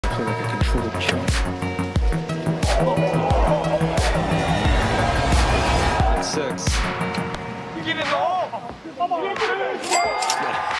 Jon Rahm short game magic
Short game highlights from Jon Rahm during the first round of the BMW Championship